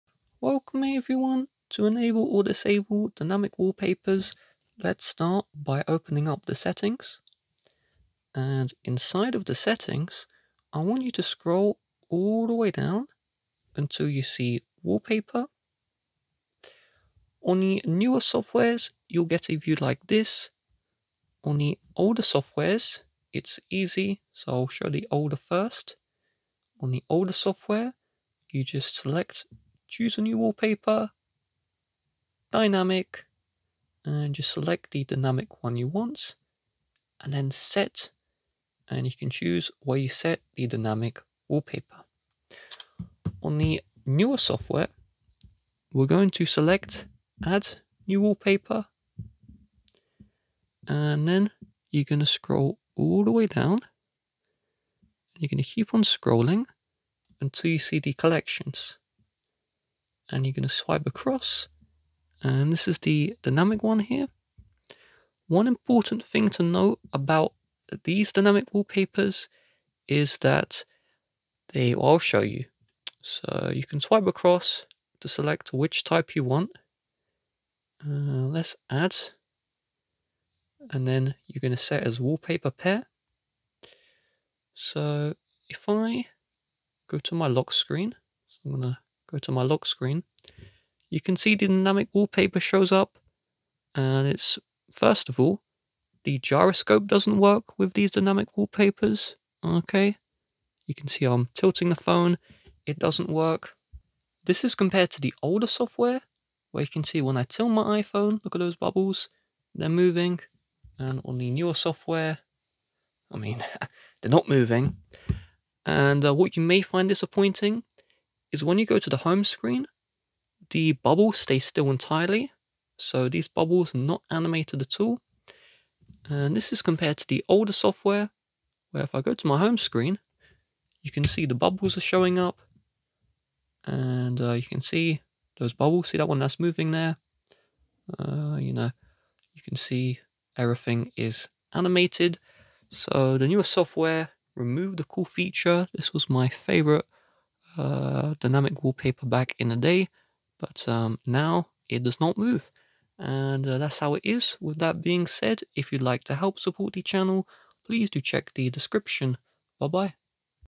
How To Enable or Disable Dynamic Wallpapers on iPhone!
Learn how to enable dynamic wallpapers on iPhone, and disable them if you'd like to as well.